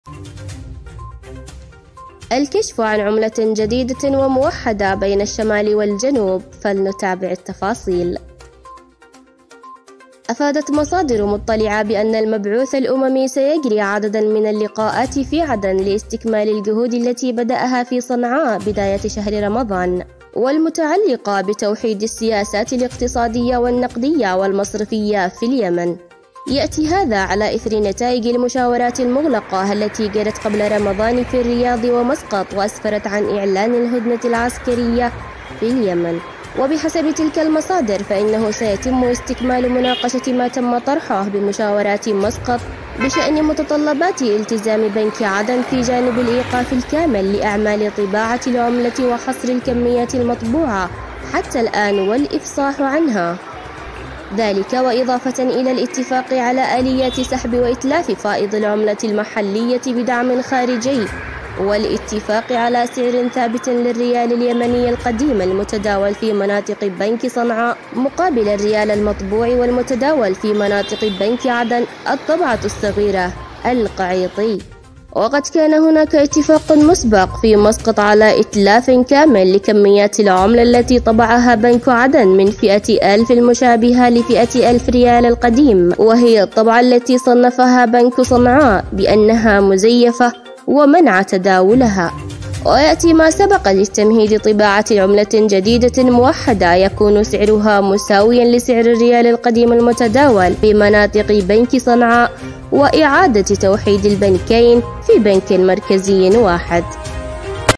الكشف عن توحيد العملة اليمنيه بين بنكي صنعاء وعدن في اليمن
الكشف عن توحيد العملة اليمنيه الجديدة والقديمه بين بنكي صنعاء وعدن في اليمن

الكشف عن عملة جديدة وموحدة بين الشمال والجنوب
أفادت مصادر مطلعة بأن المبعوث الأممي سيجري عدد من اللقاءات في عدن لاستكمال الجهود التي بدأها في صنعاء بداية شهر رمضان، والمتعلقة بتوحيد السياسات الاقتصادية والنقدية والمصرفية في اليمن. يأتي هذا على أثر نتائج المشاورات المغلقة التي جرت قبل رمضان في الرياض و#مسقط وأسفرت عن إعلان الهدنة العسكرية في اليمن

وبحسب تلك المصادر، فإنه سيتم استكمال مناقشة ما تم طرحه بمشاورات مسقط بشأن متطلبات التزام بنك عدن في جانب الإيقاف الكامل لأعمال طباعة العملة وحصر الكميات المطبوعة حتى الآن والافصاح عنها

ذلك إضافةً إلى الاتفاق على آليات سحب واتلاف فائض العملة المحلية بدعم خارجي، والاتفاق على سعر ثابت للريال اليمني القديم المتداول في مناطق بنك صنعاء مقابل الريال المطبوع والمتداول في مناطق بنك عدن (الطبعة الصغيرة - القعيطي)

ووفق المصادر ذاتها، فإن السعر الذي يتم النقاش عليه هو ريال قديم مقابل أثنين ريال ونصف من النقد المطبوع عبر بنك عدن (1 ريال قديم = 2.50 ريال قعيطي)

وقد كان هناك اتفاق مسبق في مسقط على إتلاف كامل كميات العملة التي طبعها بنك عدن من فئة 1000 المشابهة لفئة 1000 ريال القديم، وهي الطبعة التي صنفها بنك صنعاء بأنها مزيفة ومنع تداولها

ويأتي ما سبق للتمهيد لطباعة عملة جديدة موحدة يكون سعرها مساوي لسعر الريال القديم المتداول بمناطق بنك صنعاء، وإعادة توحيد البنكين في بنك مركزي واحد

توجيهات رئاسيه بفتح مطار صنعاء
واعلان رسمي بفتح مطار صنعاء الدولي وهل الجواز اليمني الصادر من صنعاء مقبول
الإعلان رسميًا عن موعد أول رحلة جوية من مطار صنعاء
أعلنت الخطوط الجوية اليمنية تسيير رحلة جوية أسبوعية، من مطار صنعاء، بدءًا من الأسبوع القادم.

وقالت الخطوط الجوية، في إعلانها ‘‘بشرى سارة لكل اليمنيين .. سيتم تسيير رحلة كل أسبوع من صنعاء إلى عمان الأردن كل يوم أحد بدءًا من الأسبوع المقبل بتاريخ 24 أبريل 2022’’.

وأشارت إلى أن هذه الخطوة جاءت ضمن تنفيذ الهدنة التي أعلنت عنها الأمم المتحدة، للتخفيف من معاناة الشعب اليمني.

بشرى سارة للمواطنين الخطوط الجوية اليمنية تعلن تسيير أولى رحلاتها من وإلى مطار صنعاء الدولي

قرارات جمهوريه جديدة في مناصب حساسه لمجلس القياده الرئاسي| ماهو سبب إغلاق خط طور الباحة عدن لحج | انتشار كثيف في خور مكسر لقوات طارق صالح تدخل عدن
مجلس القيادة الرئاسي يصدر قرارات مفاجئة وتغييرات جديدة تطال شخصيات بارزة في مناصب حساسة بالدولة (أسماء +مناصب)
أصدر رئيس مجلس القيادة الرئاسي اليمني رشاد العليمي قرارا رئاسيا جديدا بتعيين شخصية جديدة لشغل منصب وزيرا للدولة.

وقرر مجلس القيادة الرئاسي منذ قليل، تعيين أحمد حامد لملس وزيرا للدولة عضوا بمجلس الوزراء.

اصدر رئيس مجلس القيادة الرئاسي الدكتور رشاد العليمي قرارا بتعيين الاستاذ أحمد حامد لملس وزيراً للدولة. عضواً لمجلس الوزراء اضافة إلى منصبه كمحافظاً للعاصمة عدن.

عدن_المجلس_الرئاسي يعقد اول لقاء له في العاصمة. المؤقته عدن.
الزحمه في الفرشة بتجاه طور الباحه بسبب إغلاق خط القبيطة لصيانه والترميم مما ادى إلى تراكم وتكدس الشاحنات بخط الفرشه طور الباحه ومنها الى هيجة العبد، ومنها الى حيفان هذا للعلم.. الصورة في الفرشه مفرق القبيطة المغلق

عيدروس الزبيدي,
الزبيدي,
عيدروس,